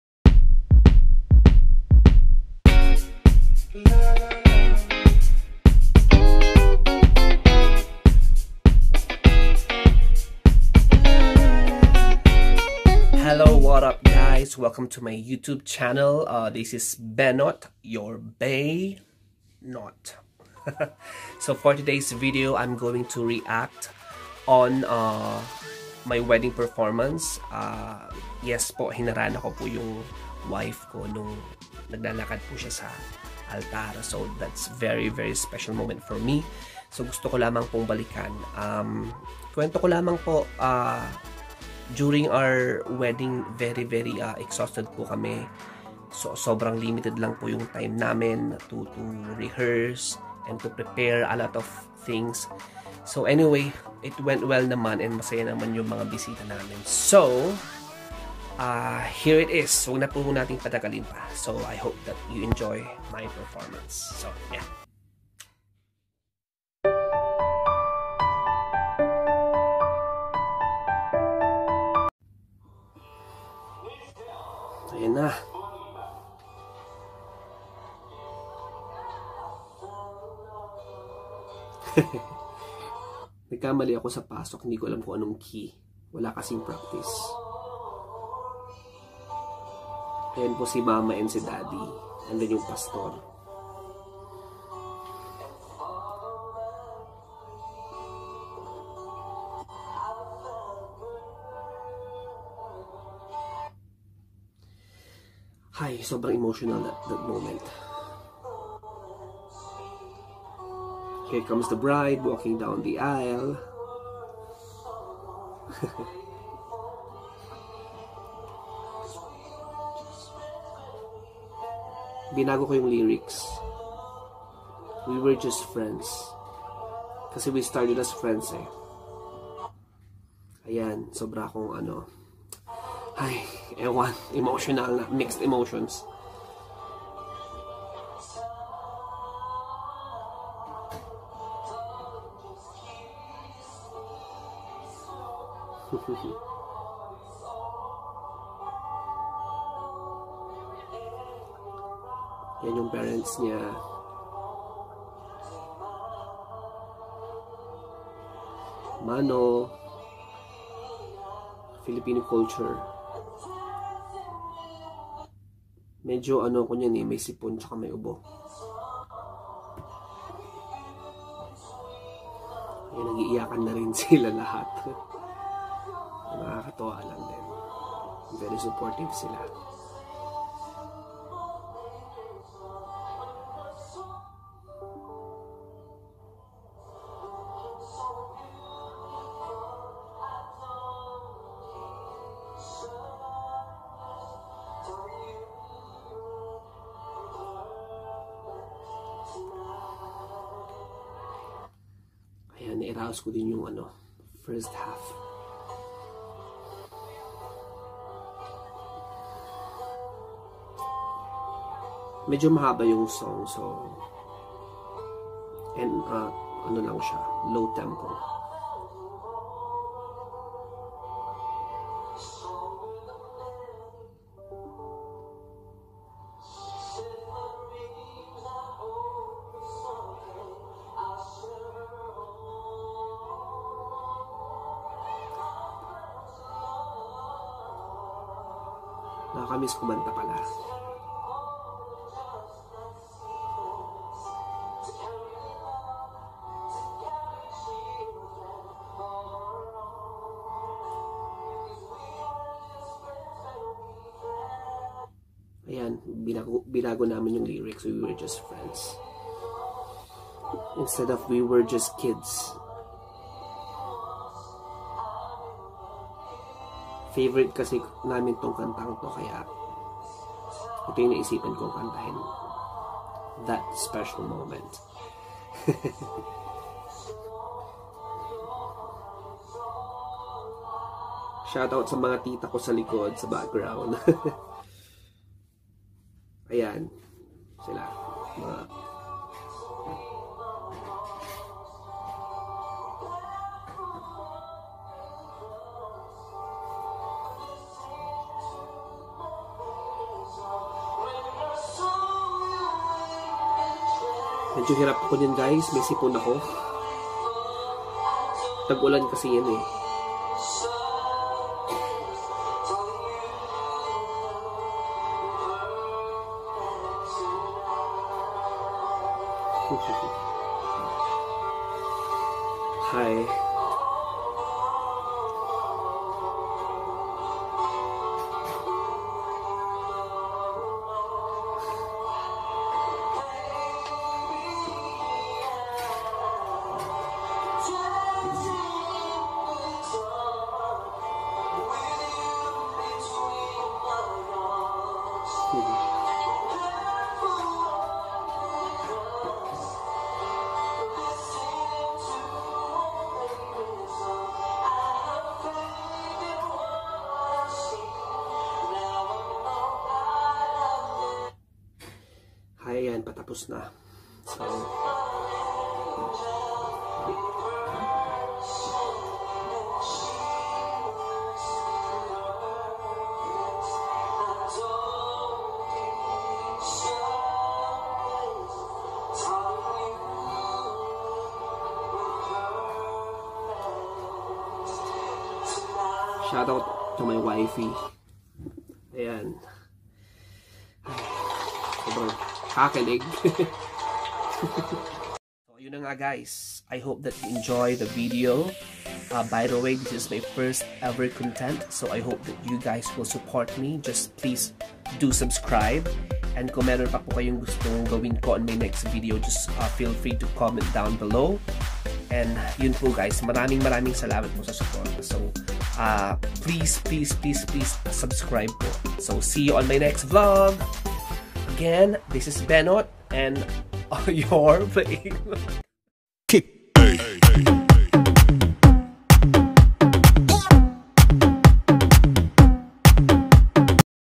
MY WEDDING PERFORMANCE REACTION | Baenot Vlogs 1
Me singing an Ed Sheeran song on our wedding day. It was indeed a very special moment and the best performance of my life ever.